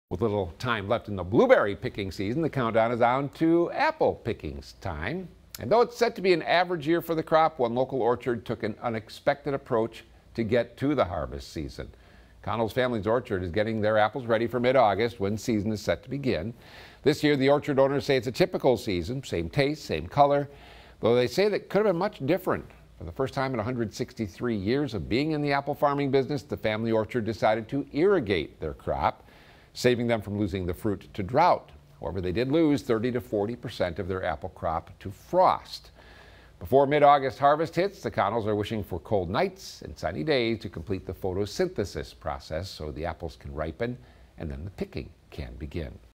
As blueberry season nears an end, apple picking is on the horizon
The countdown is on to apple picking season, and though it looks to be an average year for the crop, one local orchard took an non-average approach for their business to get to the harvest season.